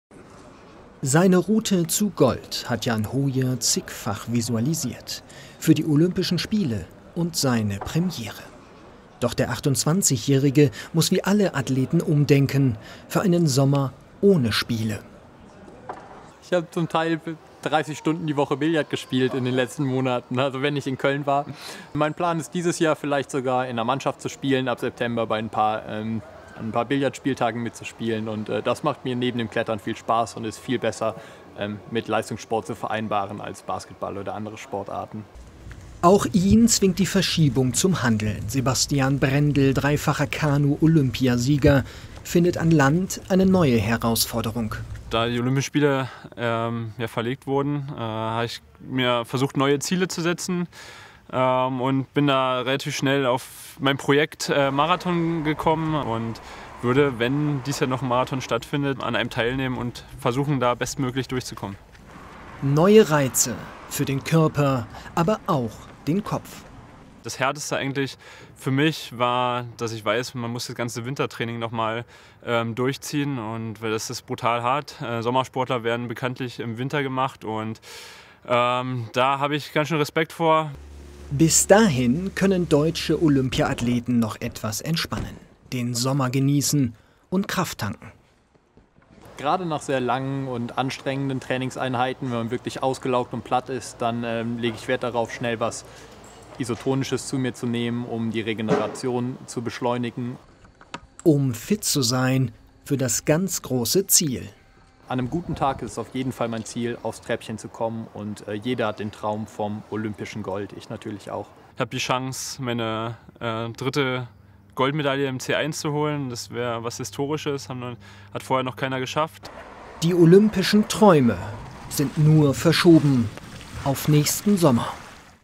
Olympia-Traum in der Warteschleife: Geduldsprobe für Brendel und Hojer
Durch die Verschiebung der Olympischen Spiele stehen die deutschen Athleten vor zahlreichen Herausforderungen in ihrer Vorbereitung. Sebastian Brendel und Jan Hojer haben die Zeit allerdings auch dazu genutzt, um sich in neuen sportlichen Feldern auszuprobieren.